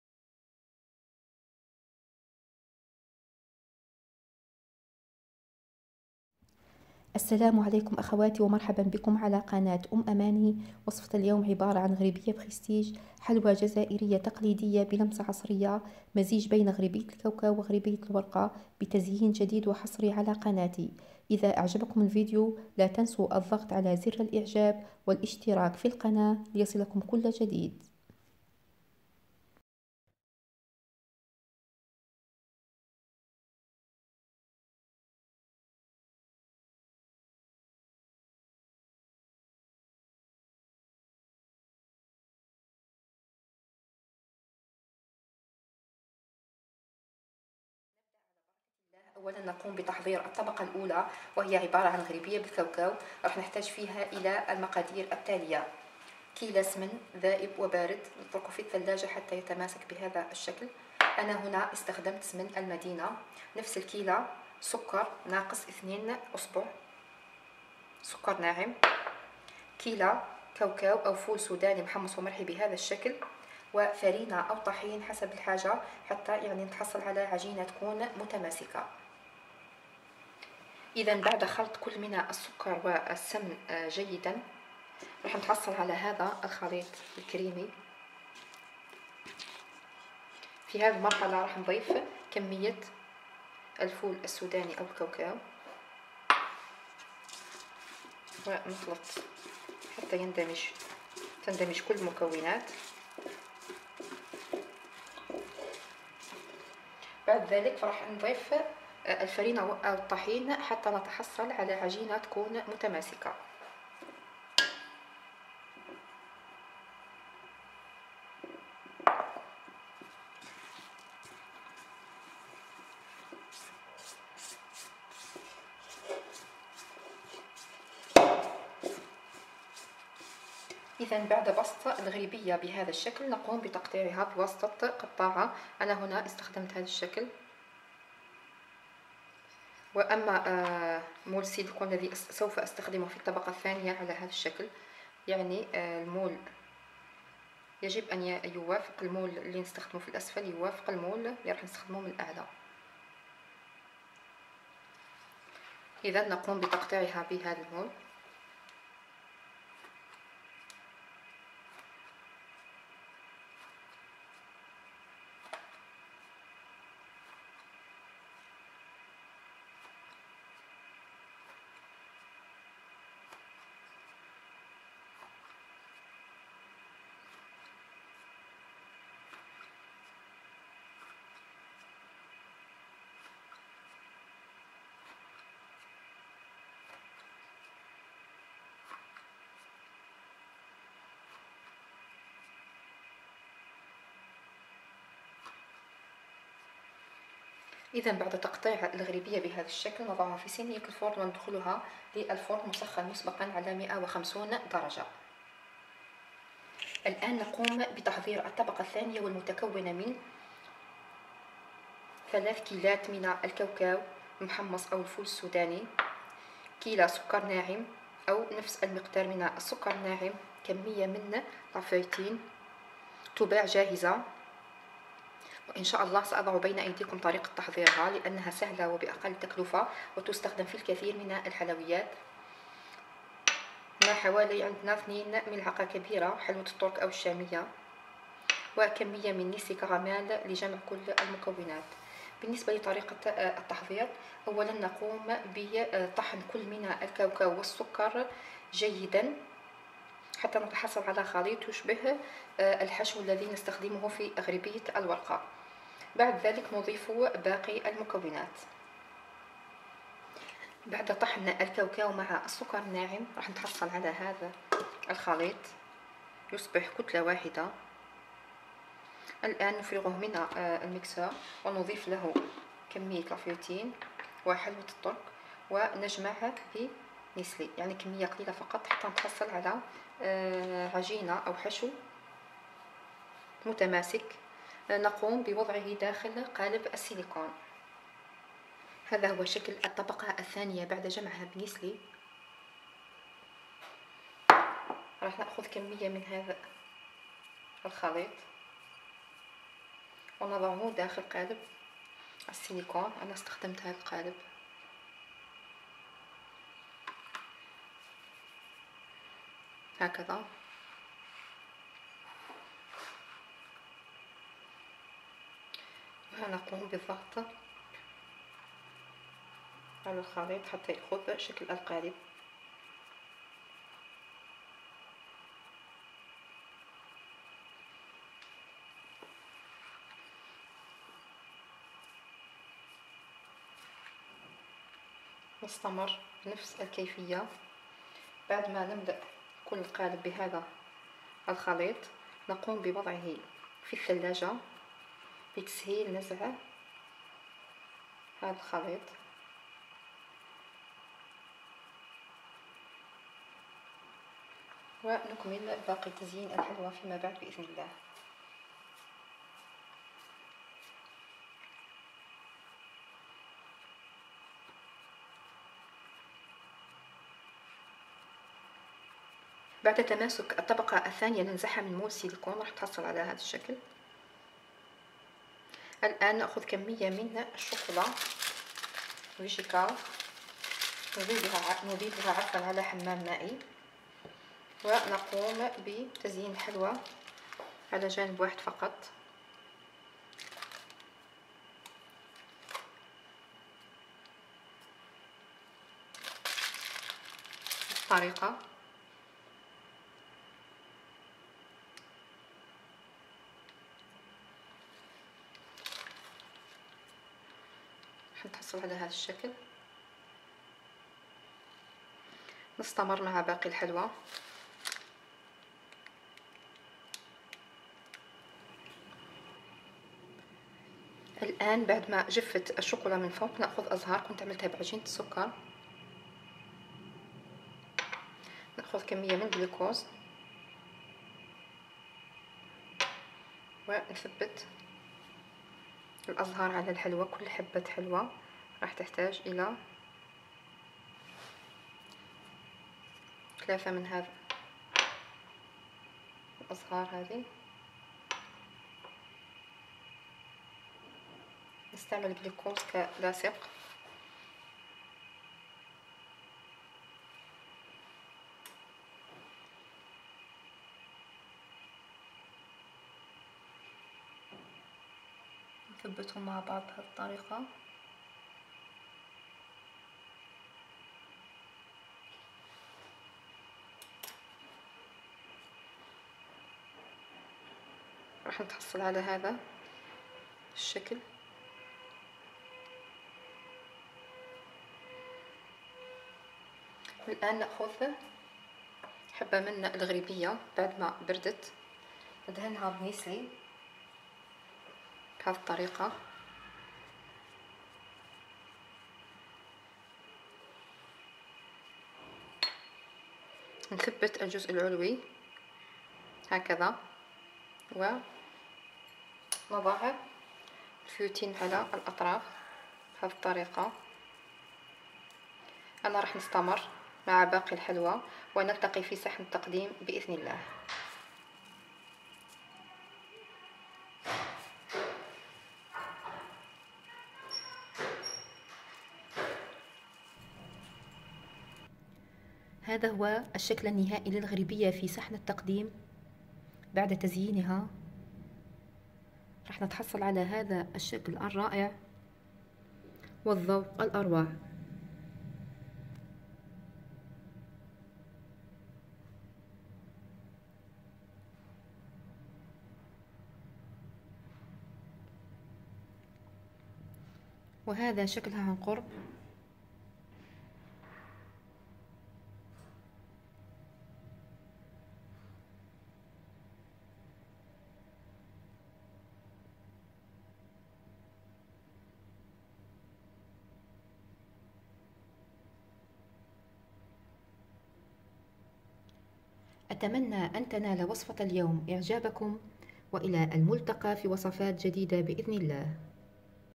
جديد حلويات 2018 غريبية بريستيج حلوى جزائرية تقليدية بلمسة عصرية حصريا على قناتي
غريبية جزائرية بطبقتين الطبقة الاولى عبارة عن غريبية الكاوكاو و الطبقة الثانية مكونة من عجينة الكاوكاو والفيوتين و حلوة الترك و نيسلي مذاق راااائع